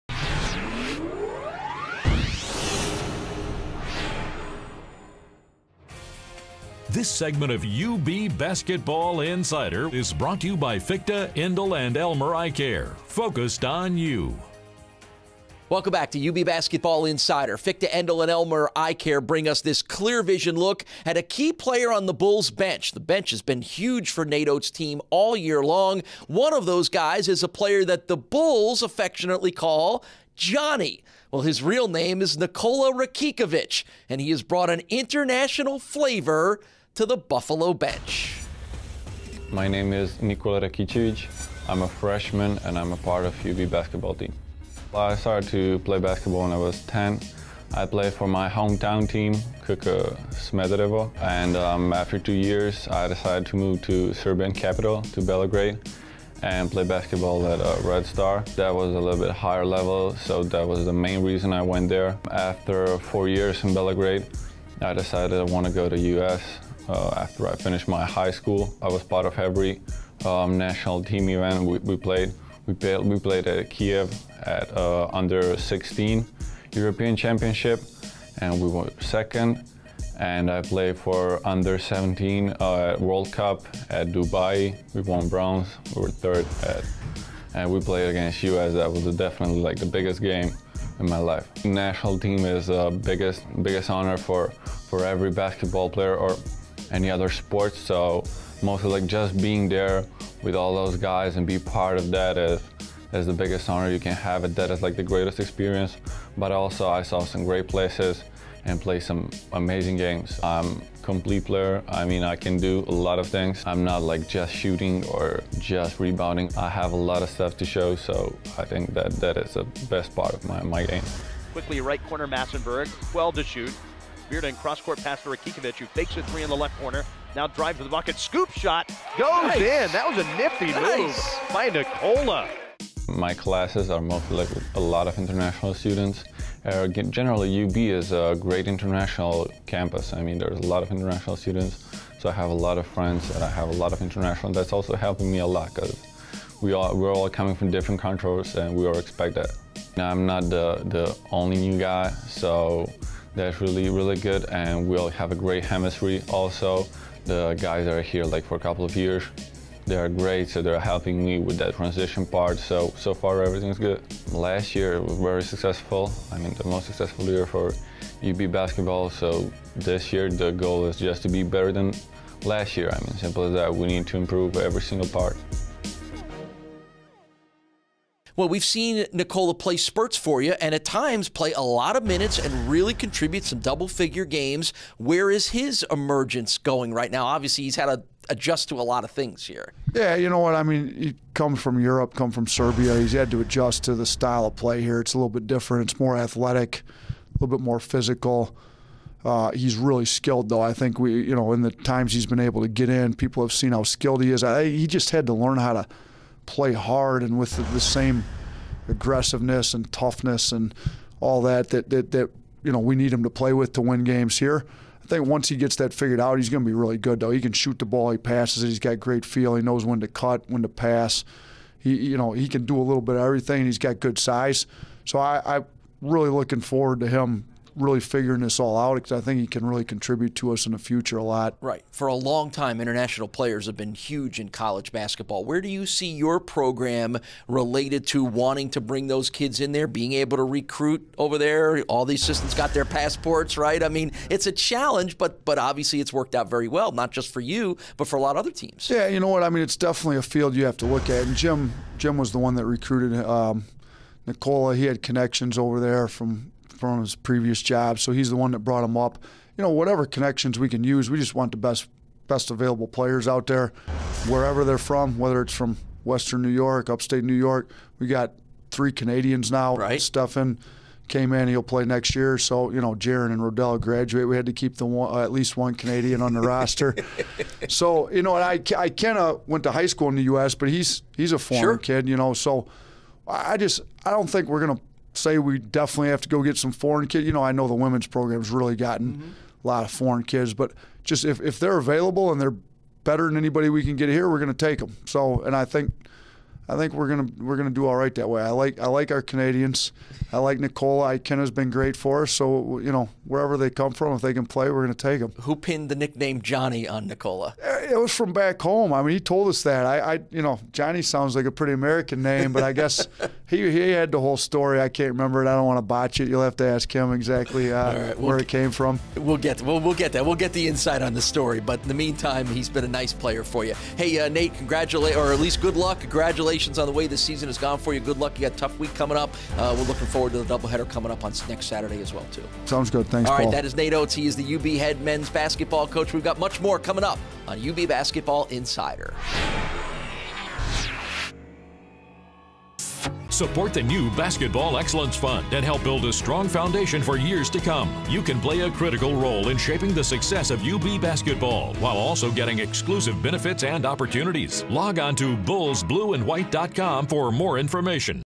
UB BASKETBALL INSIDER EP 109: Segment 2
A profile of UB Men's Basketball Player Nikola Rakicevic.